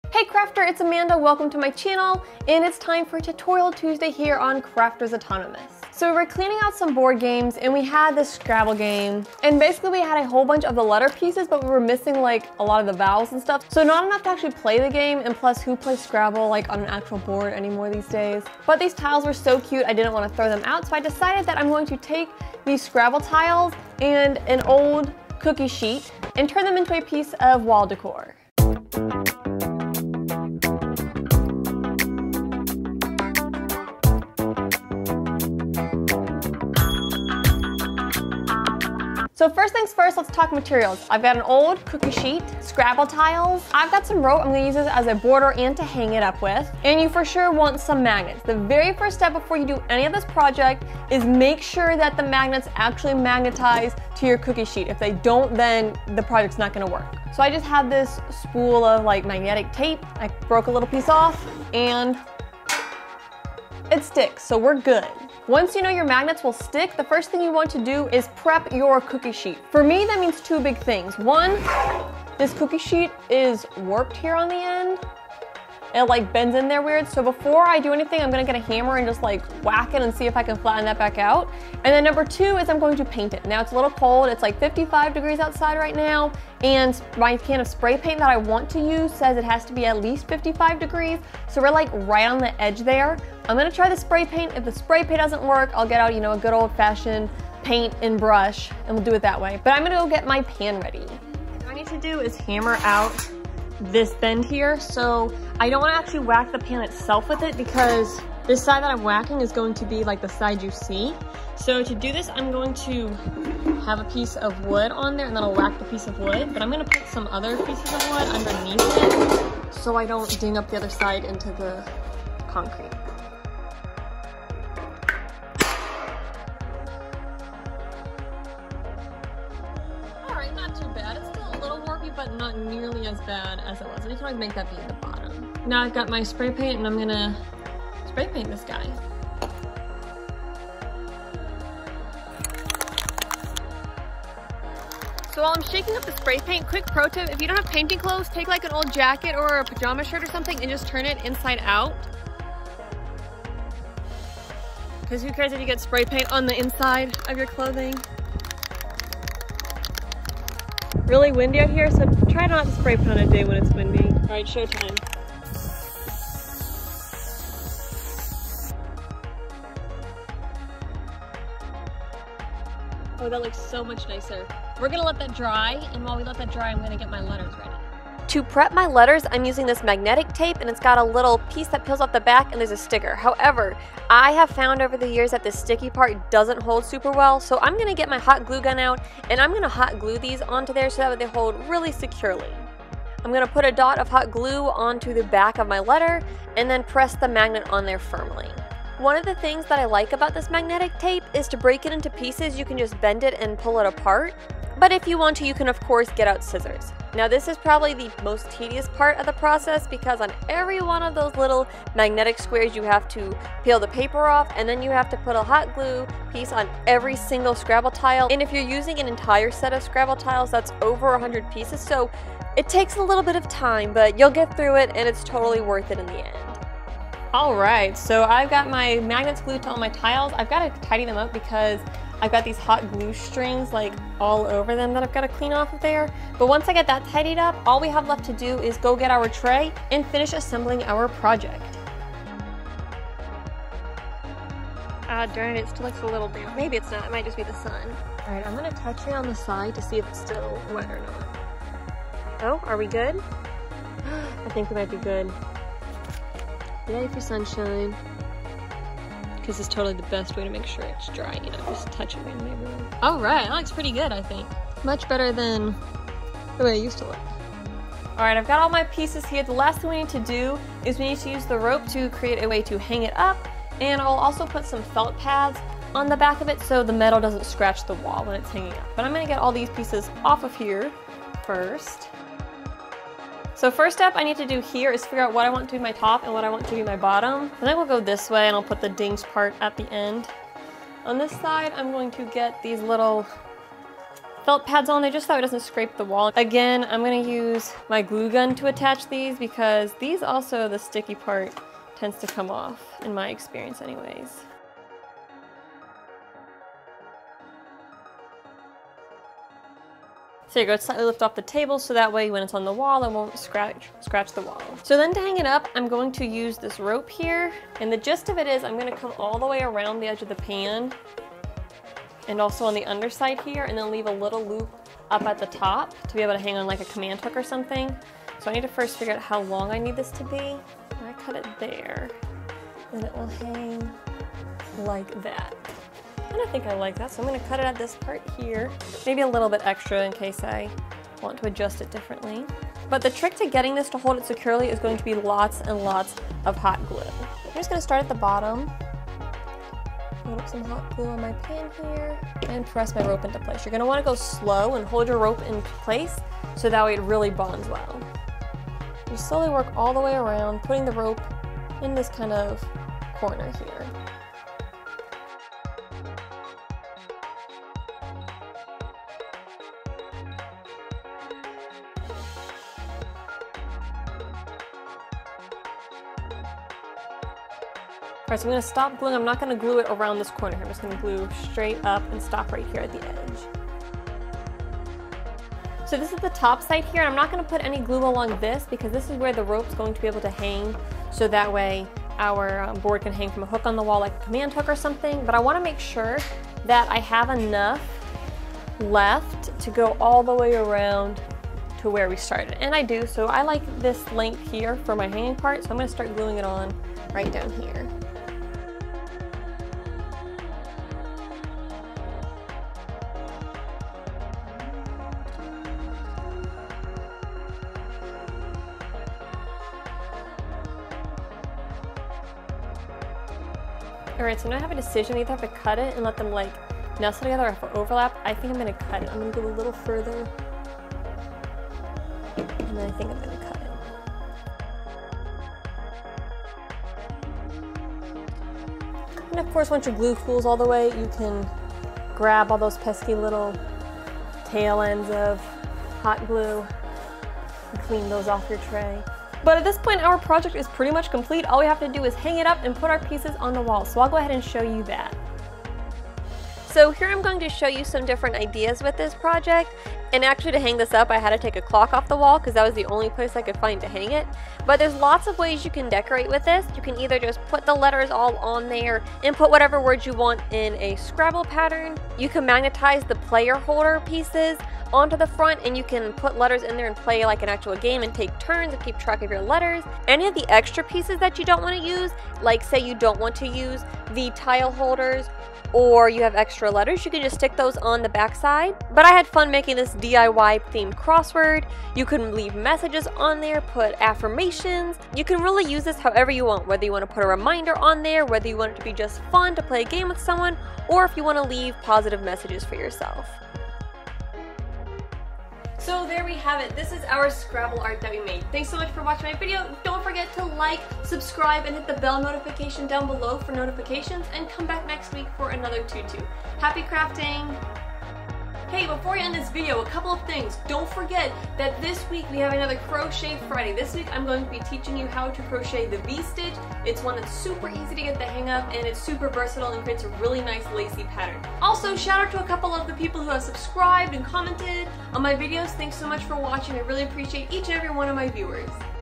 DIY Scrabble Wall Art | Tutorial Tuesday Ep. 109
Recycle your old Scrabble tiles into a fun piece of decor! This DIY scrabble decor isn't just cute. It's also functional! Leave notes, reminders, or affirmations on the board, or even play a classic game of scrabble. I've also worked storage into this project, so you can hide your extra pieces when not in use.

Will you use this Scrabble wall art to leave notes, play a game, or create a word cloud?

Rustoleum Spray Paint - Hammered Finish - Color Dark Bronze
Scrabble Game
Scrabble Tiles
Surebonder High-Temp Mini Glue Gun

Filmed with:
Canon Rebel T7i
Canon 10-18 STM Lens
BOYA Lavalier Mic